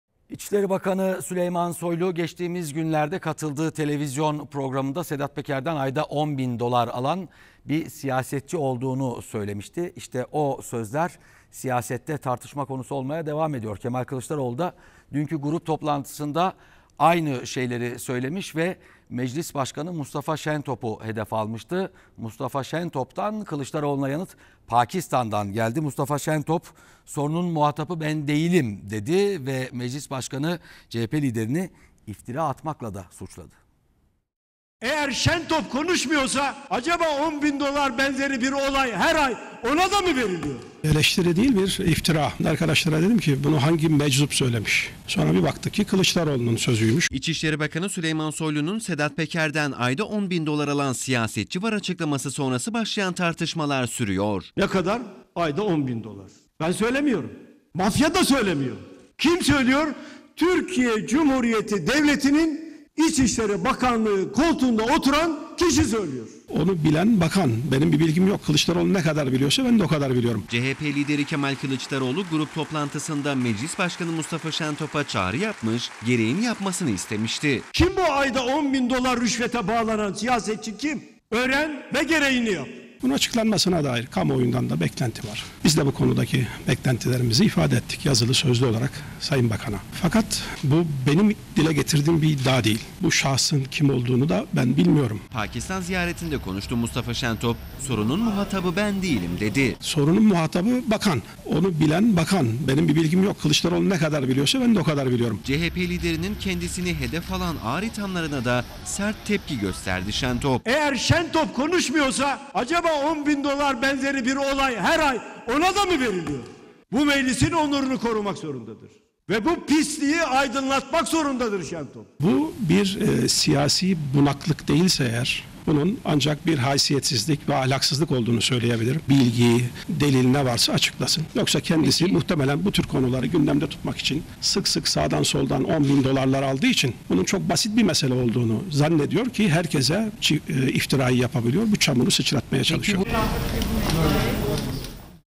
TBMM Başkanı Şentop'tan Kılıçdaroğlu'na tepki
TBMM Başkanı Mustafa Şentop, CHP lideri Kemal Kılıçdaroğlu'nun kendisine yönelik sözlerine "Bu bir eleştiri değil bir iftira" diyerek cevap verdi.